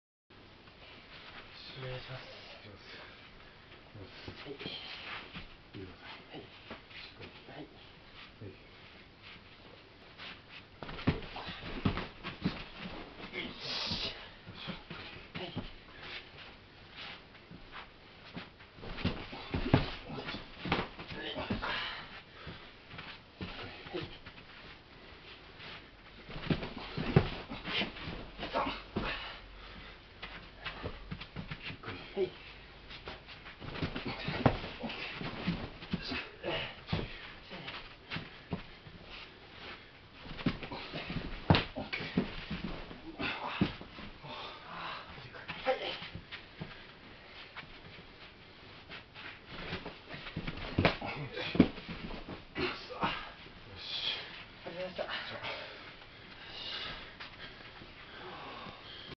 【26.12.19】連絡技：小外刈り、大内刈り、大外刈り
お疲れ様です。マッスルサトーです。連絡技でございます。小外刈り、大内刈り、大外刈りです。よろしければご覧くださいませ。